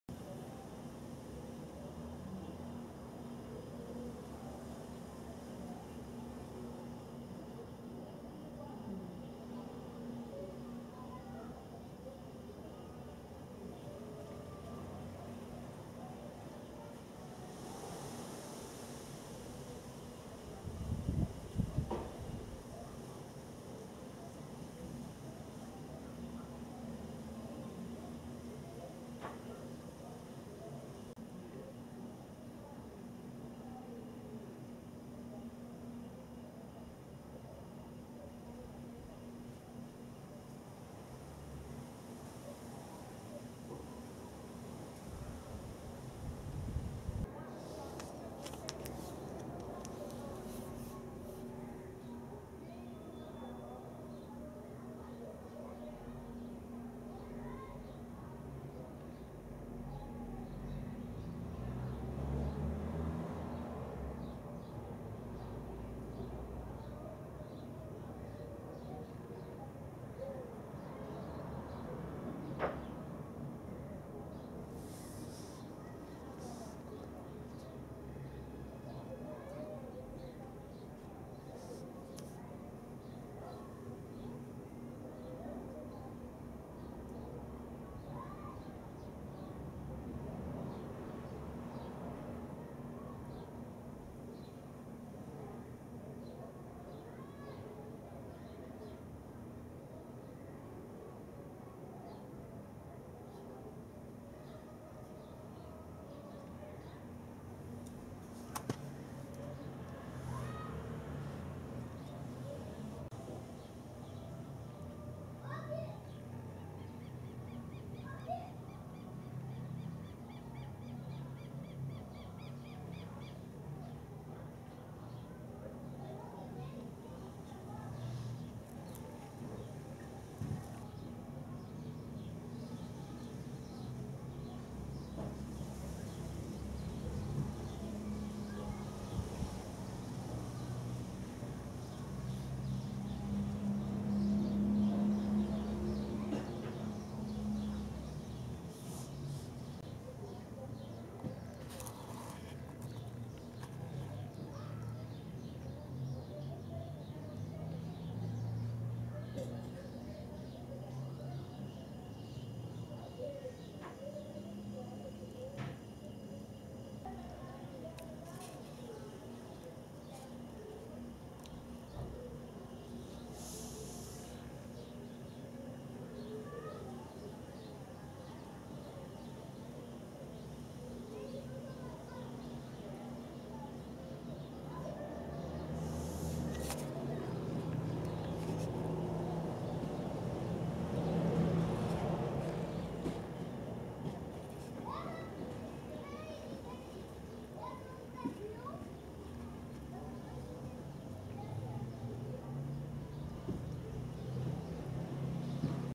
Taturana (Megalopyge lanata)
também conhecido como mandrová, lagarta de fogo ou simplesmente lagarta, é o estágio larval da mariposa